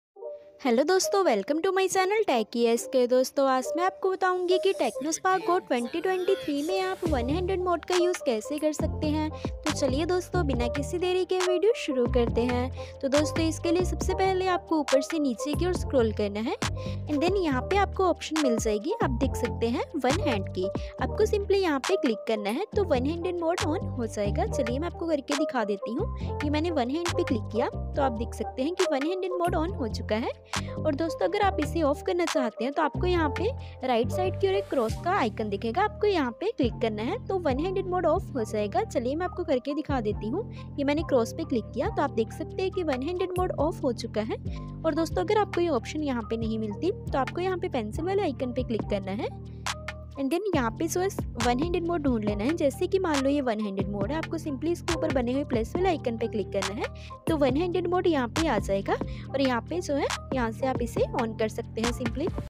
How to enable one handed mode in Tecno Spark Go 2023
Hello dosto, aaj maine is video me bataya hai ki Tecno Spark Go 2023 me one handed mode kaise enable kare, dosto Tecno Spark Go 2023 me one handed mode enable karna bahut aasan hai. Agar aap bhi janna chahate hain ki Tecno Spark Go 2023 me one handed mode kaise enable kare to is video ko pura dekhiyega, thanks friends.